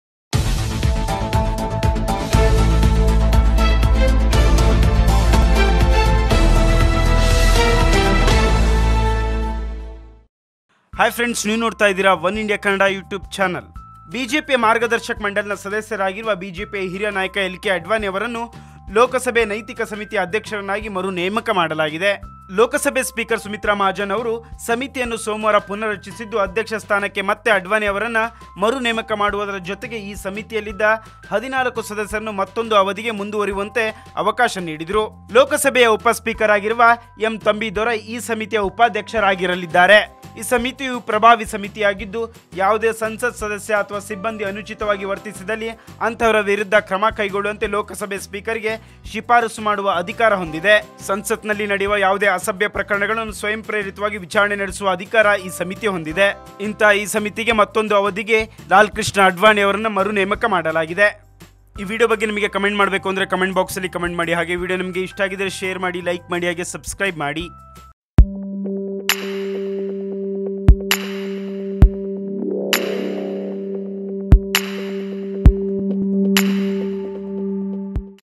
Advani Renominated As Lok Sabha Ethics committee | Oneindia Kannada
BJP Margdarshak Mandal member and veteran leader L K Advani has been renominated as Chairman of the Lok Sabha Ethics Committee by Speaker Sumitra Mahajan. Advani and all other 14 members have been given another term.
ಬಿಜೆಪಿಯ 'ಮಾರ್ಗದರ್ಶಕ್ ಮಂಡಲ್' ನ ಸದಸ್ಯರಾಗಿರುವ ಬಿಜೆಪಿಯ ಹಿರಿಯ ನಾಯಕ ಎಲ್.ಕೆ. ಅಡ್ವಾಣಿಯವರನ್ನ ಲೋಕಸಭೆ ನೈತಿಕ ಸಮಿತಿಯ ಅಧ್ಯಕ್ಷರನ್ನಾಗಿ ಮರು ನೇಮಕ ಮಾಡಲಾಗಿದೆ.